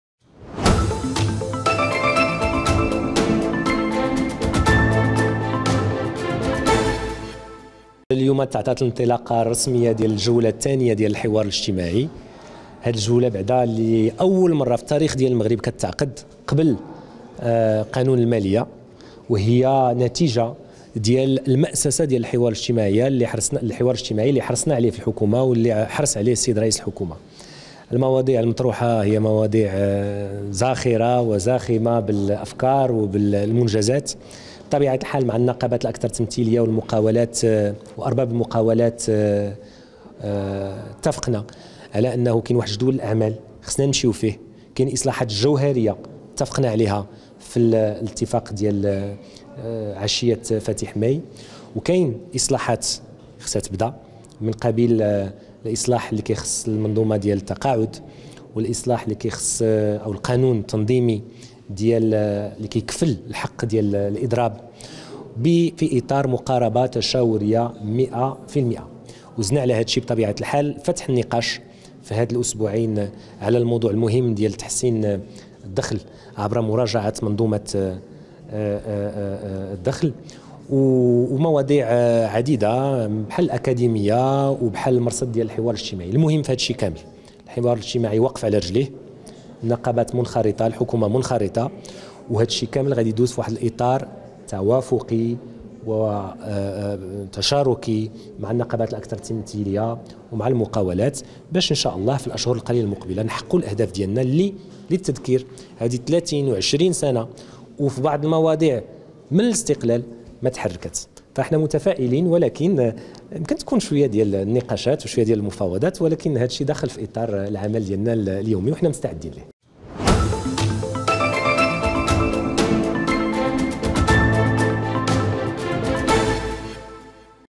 الوزير السكوري: إصلاح منظومة التقاعد والحق في الإضراب من بين نقط الحوار الاجتماعي
تم الاتفاق خلال المحطة الثانية من جولات الحوار الاجتماعي، على تشكيل لجان مشتركة لمعالجة المواضيع الراهنة، وفق الجدولة الزمنية المبرمجة في اتفاق 30 أبريل 2022، تهم بالخصوص مراجعة نظام الضريبة على الدخل، وإصلاح أنظمة التقاعد، وإعادة النظر في عدد  من تشريعات العمل

# أخبار_عاجلة